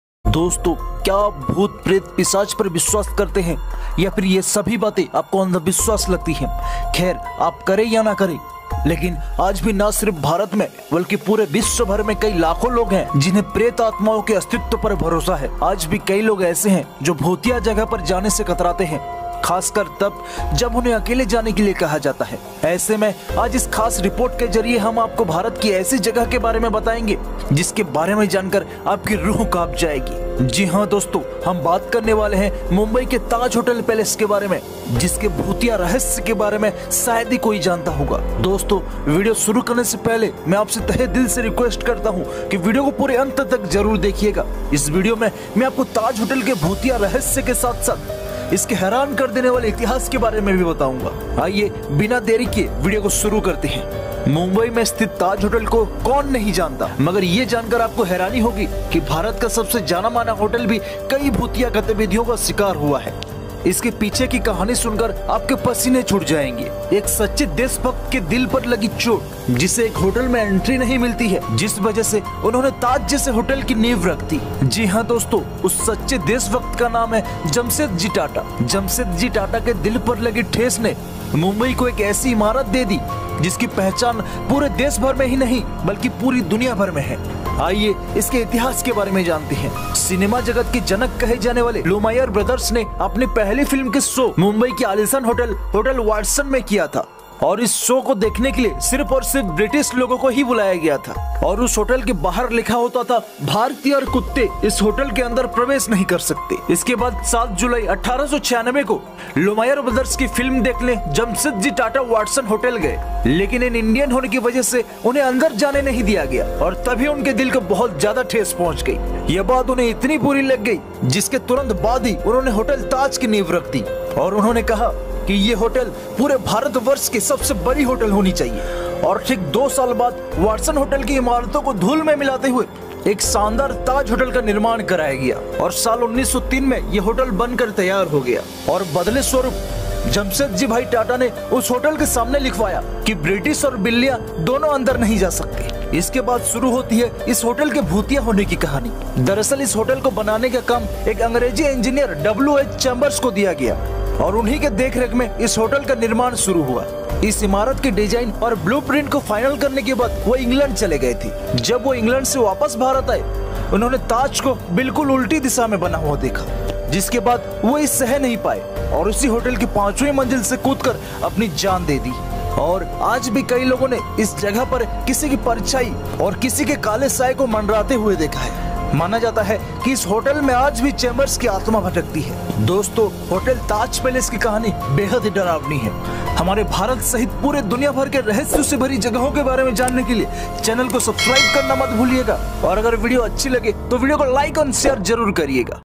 Hotel Taj Palace||Hotel Taj Palace का रहस्य||Hotel Taj Palace का भूतिया रहस्य||Hotel Taj का इतिहास
दोस्तों आज हम आपको इस वीडियो मैं Hotel Taj Palace ka rahasya,Hotel Taj ka bhutiya rahasya, hotel Taj ka itihas in sab ke bare main bataenge

The Taj Mahal Palace is a heritage, five-star, luxury hotel in the Colaba area of Mumbai, Maharashtra, India, situated next to the Gateway of India. Built in the Indo-Saracenic style, it opened in 1903 as the Taj Mahal Hotel and has historically often been known simply as "The Taj". The hotel is named after the Taj Mahal, which is located in the city of Agra approximately 1,050 kilometres (650 mi) from Mumbai. It has been considered one of the finest hotels in the East since the time of the British Raj. The hotel was one of the main sites targeted in the 2008 Mumbai attacks.

Part of the Taj Hotels Resorts and Palaces, the hotel has 560 rooms and 44 suites and is considered the flagship property of the group; it employs 1,600 staff. The hotel is made up of two different structures: the Taj Mahal Palace and the Tower, which are historically and architecturally distinct from each other (the Taj Mahal Palace was built in 1903; the Tower was opened in 1972).

The hotel has a long and distinguished history, having received many notable guests, from presidents to captains of industry and show business stars.

मुंबई की कोलाबा नामक जगह पर स्थित ताज महल पैलेस होटल पांच सितारा होटल है जो कि गेटवे ऑफ़ इंडिया के पास है। ‘ताज होटल, रिसॉर्ट्स एंड पैलेस’ का एक हिस्सा, यह इमारत इस समूह की प्रमुख संपत्ति मानी जाती है, जिसमे ५६० कमरे एवं ४४ सुइट्स हैं।